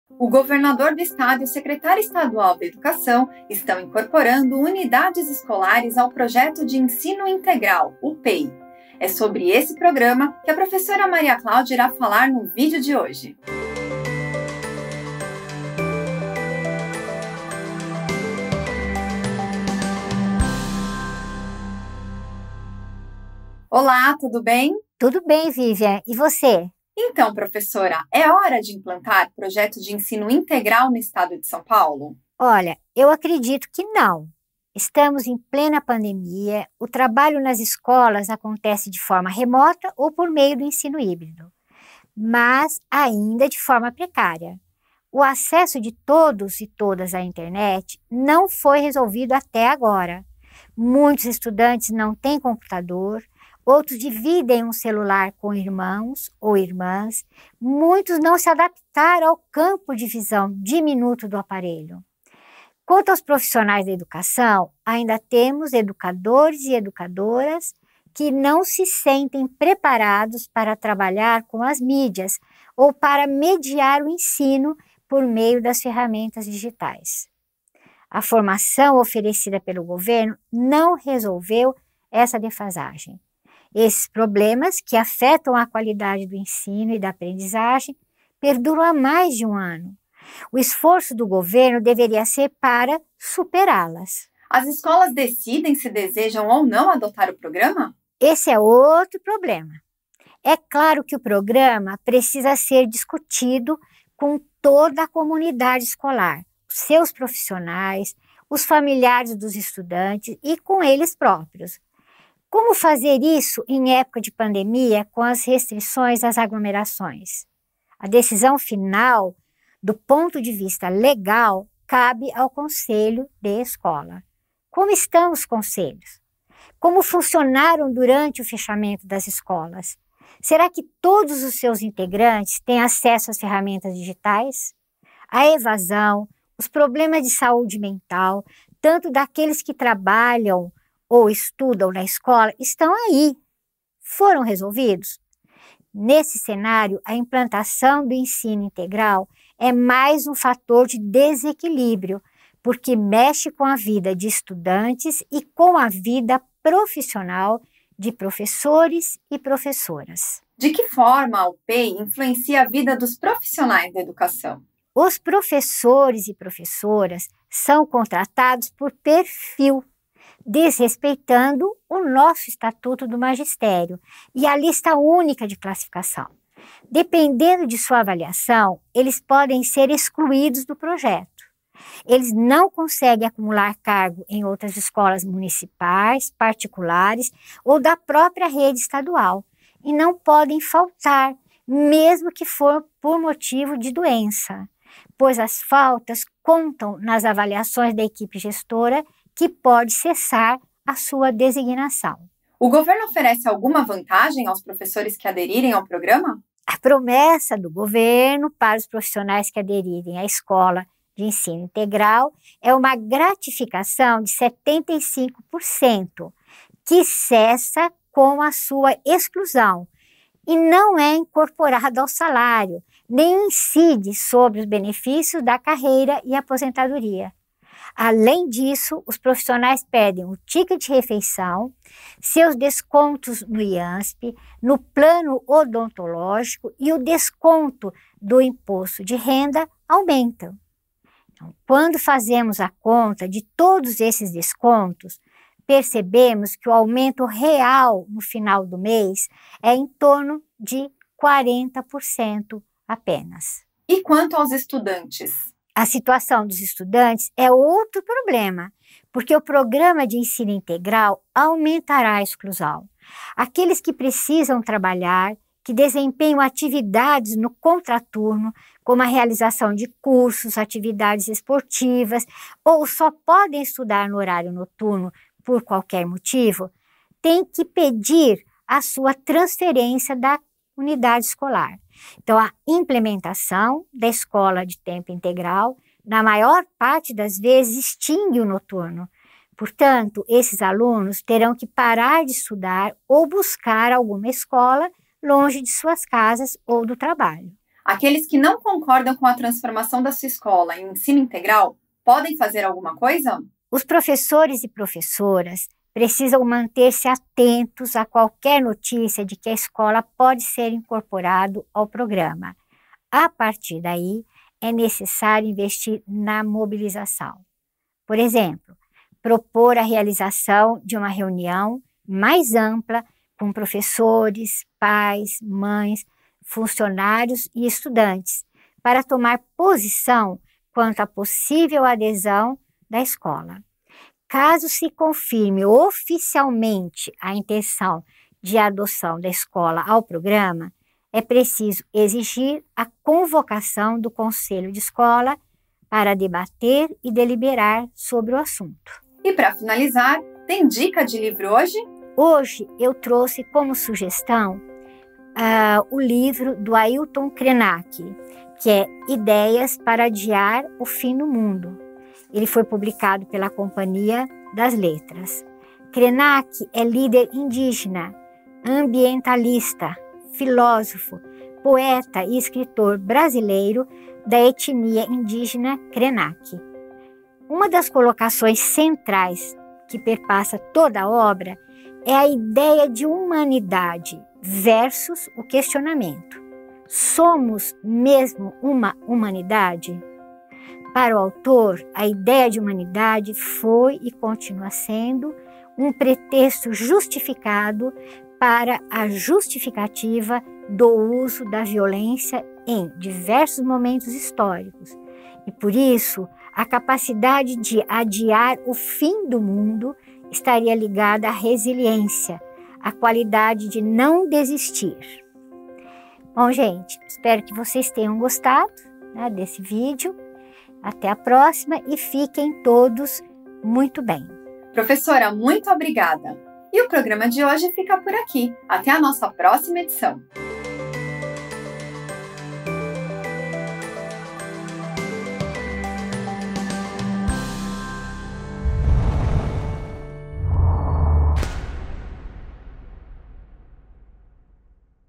A polêmica da implantação do PEI no estado de SP
No programa de hoje, a professora Maria Claudia esclarece algumas dúvidas sobre o Projeto de Ensino Integral do governo estadual. Confira!